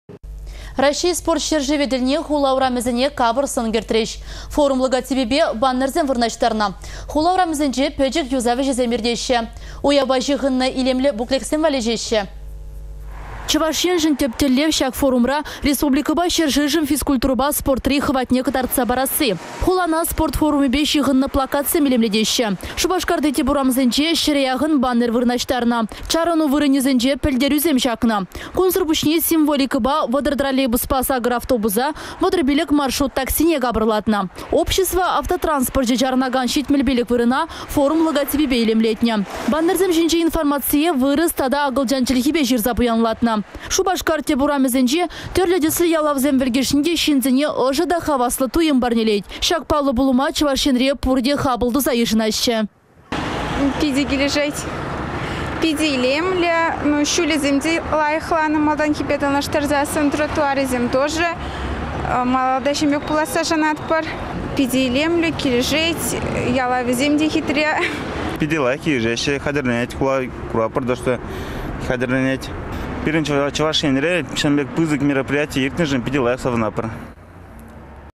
Сумлă мероприяти умĕн Шупашкар капăр тум тăхăннă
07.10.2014
«Раççей-спорт çĕр-шывĕ» форум тĕлне хула урамĕсене капăр сăн кĕртрĕç. Форум логотипĕпе баннерсем вырнаçтарнă, хула урамĕсенче пĕчĕк юсав ĕçĕсем иртеççĕ, уяпа çыхăннă илемлĕ буклетсем валеçеççĕ.